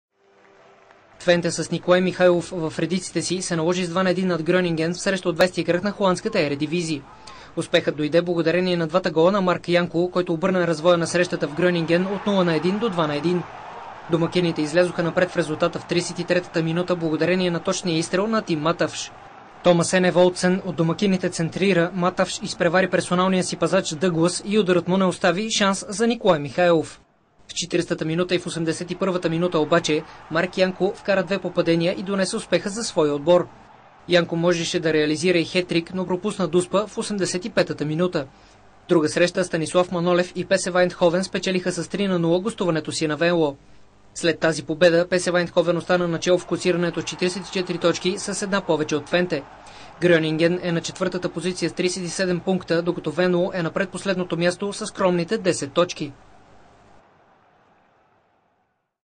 Mihaylov and Twente against Groningen
Video: EvropaTV
Twente beats Groningen 2:1